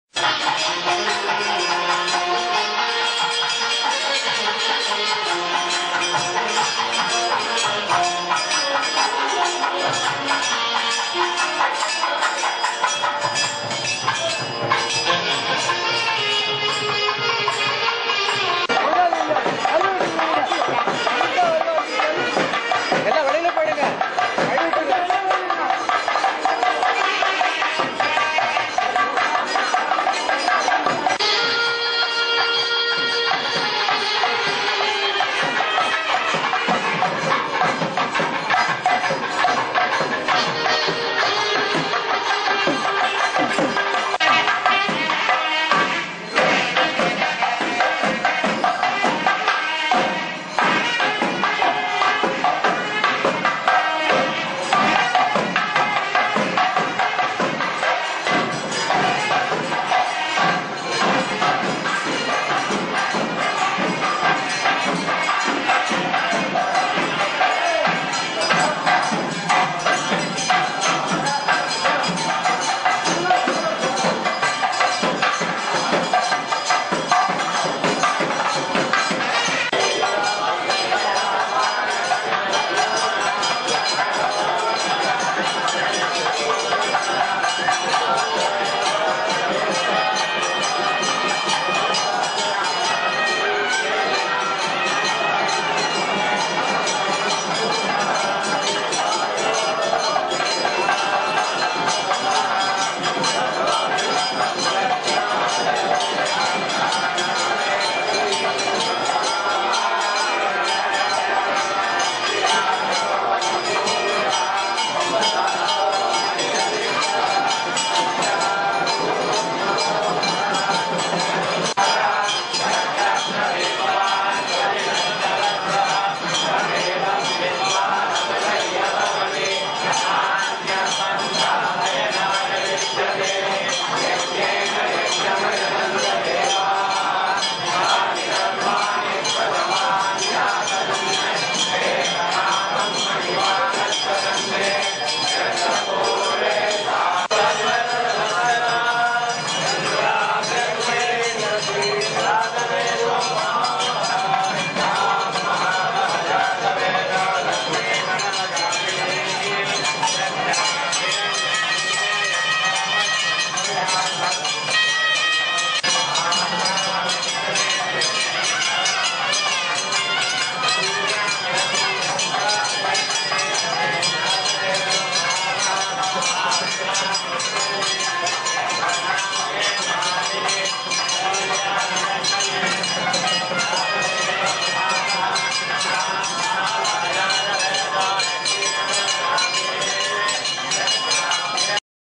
Pushpa Yagam Sri Lakshmi Nrisimhar Sri Mahalakshmi Sannidhi Vidayathi Sri Nrisimha Jayanthi
Sri Ahobila Mutt Temple Chembur Mumbai